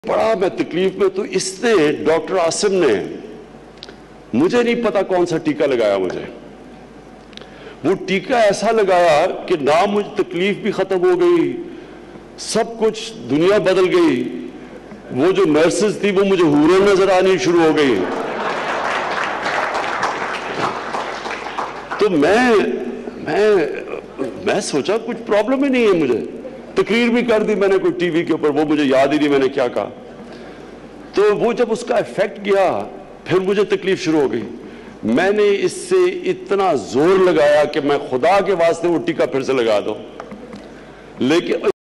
Injection lagne kay bad Nursain Horain Lagnay Lag Gayen
انجکشن لگنے کے بعد اسپتال کی نرسیں حوریں لگنے لگیں،وزیراعظم عمران خان